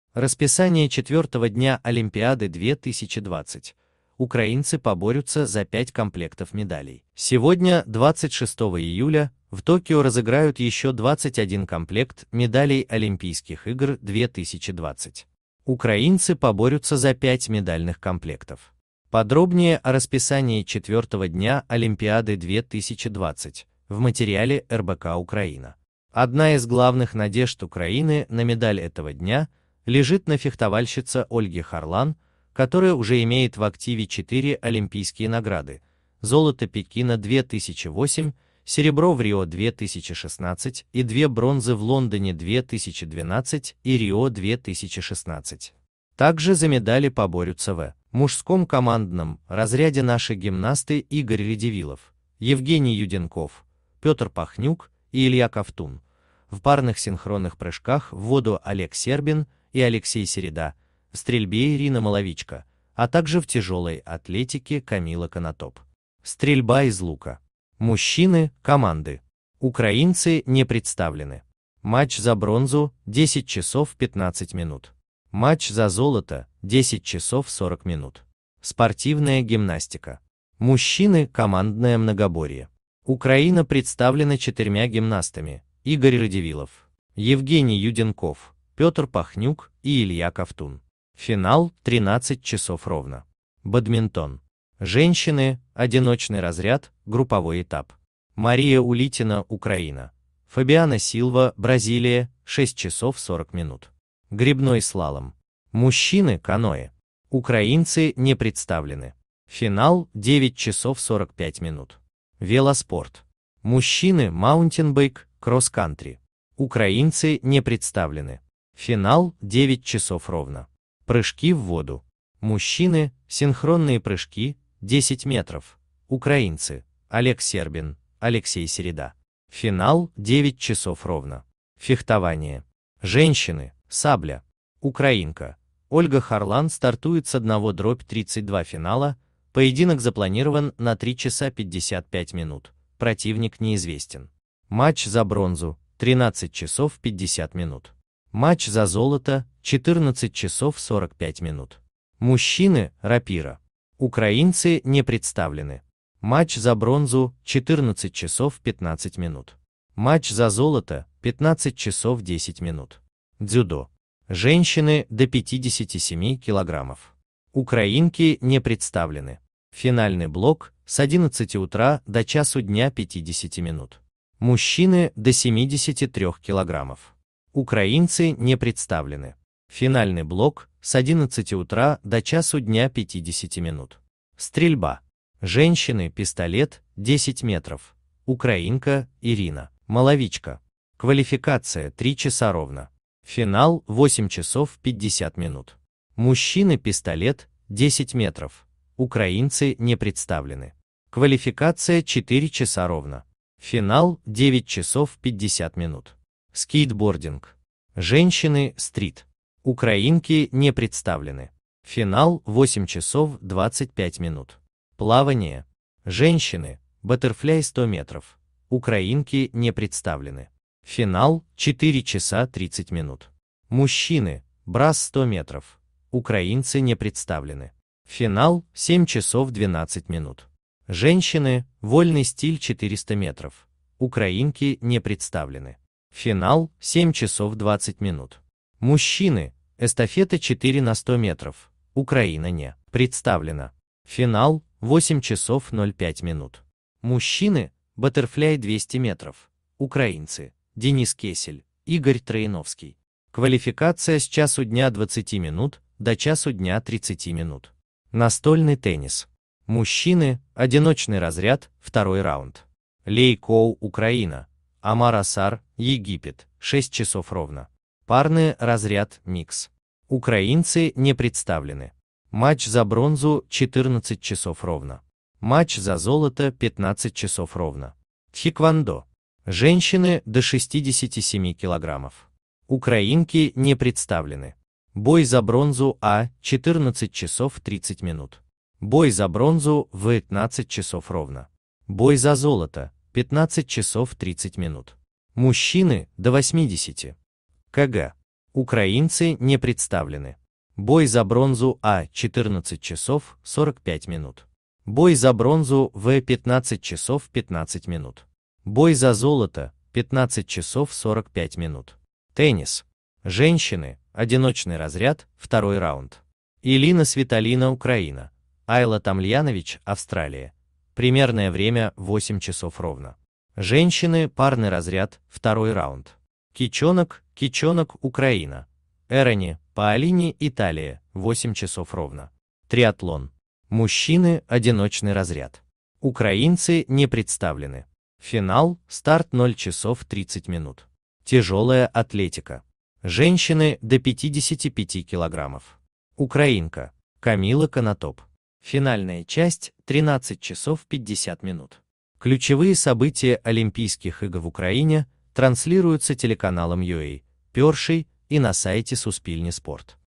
Расписание четвертого дня Олимпиады-2020: украинцы поборются за пять комплектов медалей.
ОльгаХарлан Олимпиада Олимпиада2020 Сегодня, 26 июля, в Токио разыграют еще 21 комплект медалей Олимпийских игр-2020. Украинцы поборются за пять медальных комплектов. Подробнее о расписании четвертого дня Олимпиады-2020 - в материале РБК-Украина. Одна из главных надежд Украины на медаль этого дня лежит на фехтовальщице Ольге Харлан, которая уже имеет в активе четыре олимпийские награды (золото Пекина-2008, серебро в Рио-2016 и две бронзы в Лондоне-2012 и Рио-2016). Также за медали поборются в мужском командном разряде наши гимнасты И